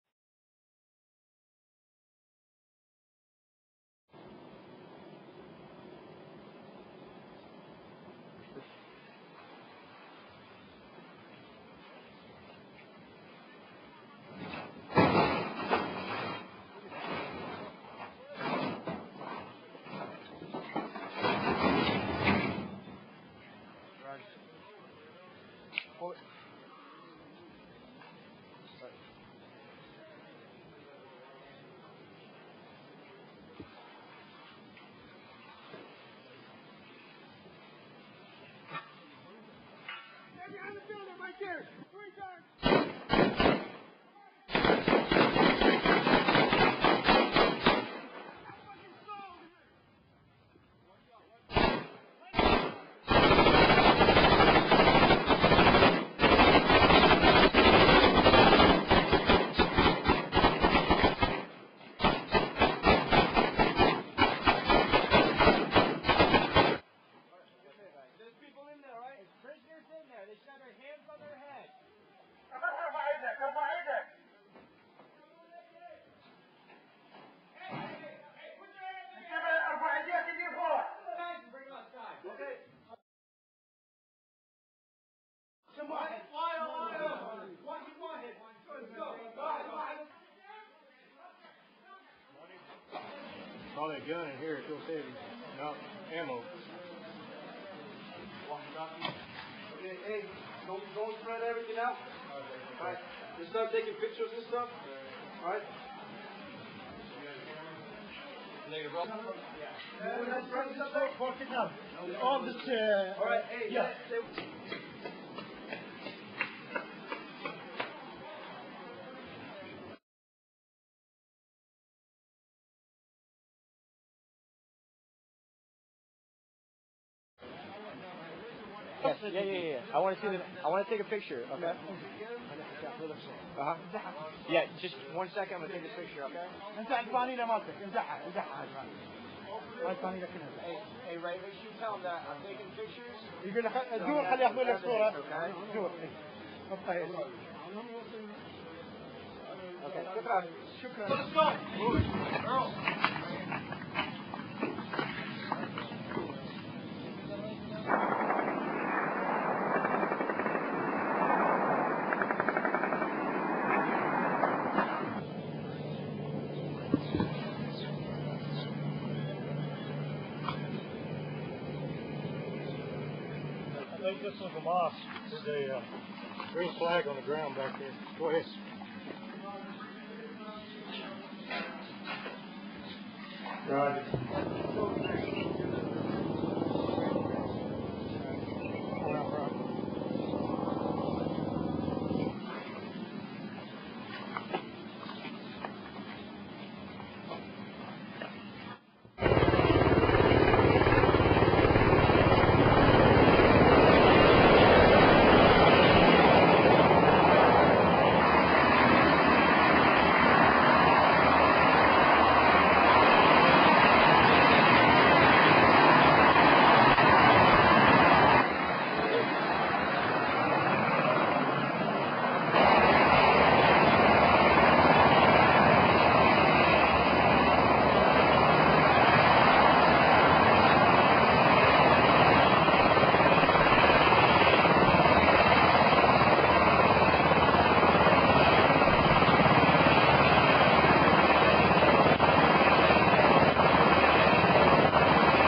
Operation Ithaca - The Fight
5th Squadron, 73rd Cavalry Regiment, 3rd brigade Team, 82nd Airborne Division, conducting raids as part of Operation Ithaca. Scenes include Soldiers searching abandoned buildings, firing rifles, questioning suspicious citizens, rescuing hostages that had been tortured by Al Qaeda and putting people on helicopters.